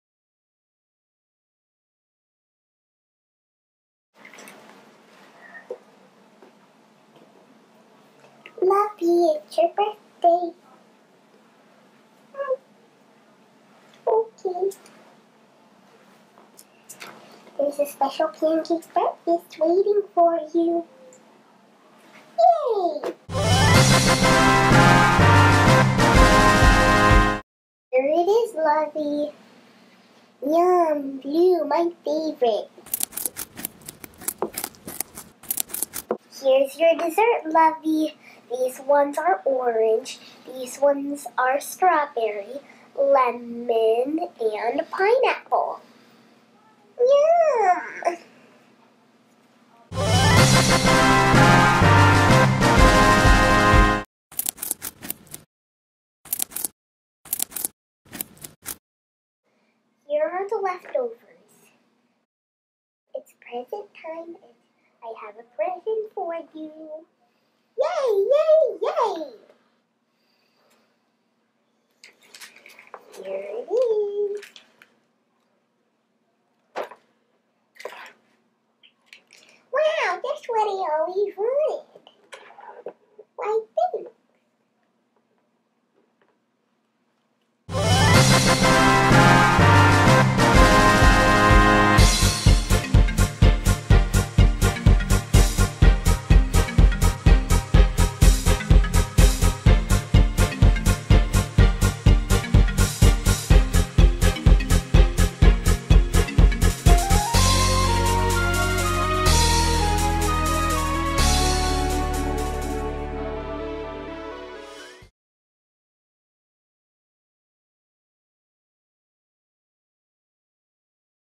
Lovey's BIRTHDAY!
Lovey receives surprises on her birthday!